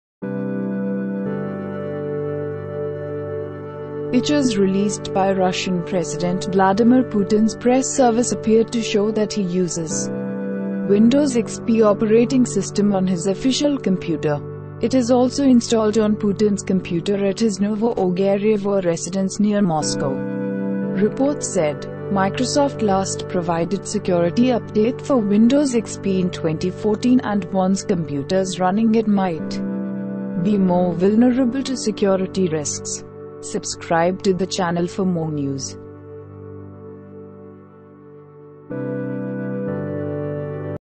Putin appears to use Windows XP which got last update in 2014
Pictures released by Russian President Vladimir Putin's press service appeared to show that he uses Windows XP operating system on his official computer. It is also installed on Putin's computer at his Novo-Ogaryovo residence near Moscow, reports said. Microsoft last provided security update for Windows XP in 2014 and warns computers running it might be more vulnerable to security risks.